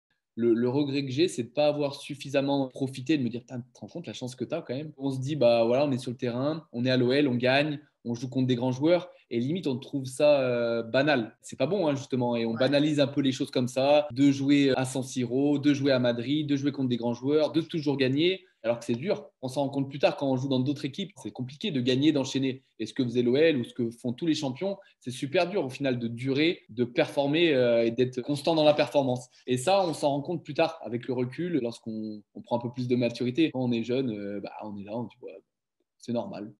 Jérémy Clément évoque le plus grand regret de sa carrière... Yoram Bismuth
Rendez-vous sur Youtube pour voir l'interview intégrale de Jérémy Clément, ancien joueur du PSG et de Lyon !